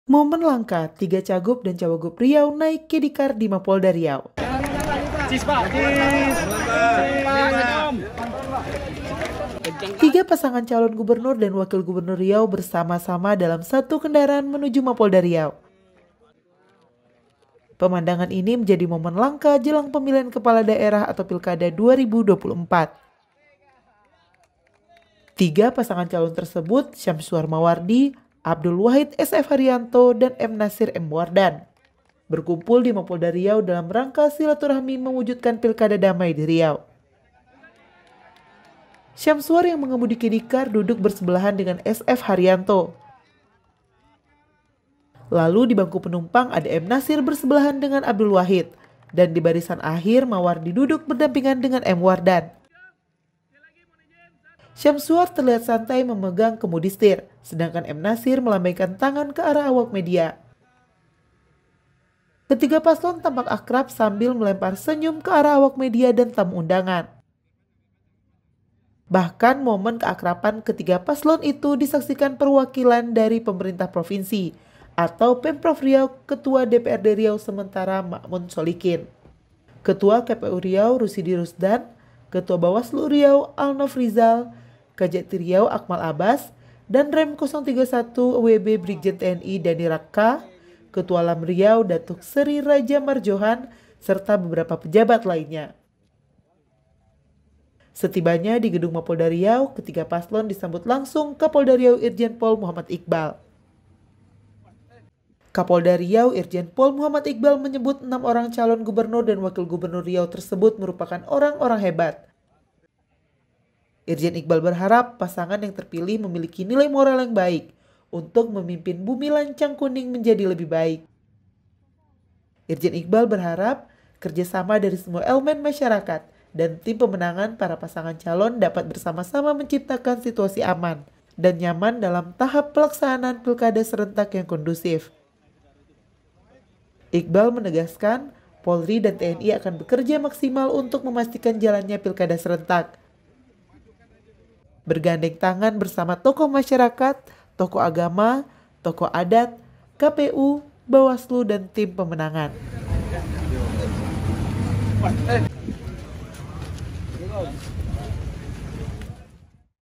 Momen Langka, Tiga Cagub dan Cawagub Riau Naik Caddy Car di Mapolda Riau
Tiga pasangan calon Gubernur dan Wakil Gubernur Riau bersama-sama dalam satu kendaraan menuju Mapolda Riau. Pemandangan ini menjadi momen langka jelang Pemilihan Kepala Daerah (Pilkada) 2024.

Tiga pasangan calon tersebut, Syamsuar-Mawardi, Abdul Wahid-SF Hariyanto, dan M Nasir-M Wardan, berkumpul di Mapolda Riau dalam rangka silaturahmi mewujudkan Pilkada damai di Riau.

Syamsuar yang mengemudikan caddy car, duduk bersebelahan dengan SF Hariyanto. Lalu di bangku penumpang, ada M Nasir bersebelahan dengan Abdul Wahid dan di barisan akhir Mawardi duduk berdampingan dengan M Wardan.

(RiauOnline)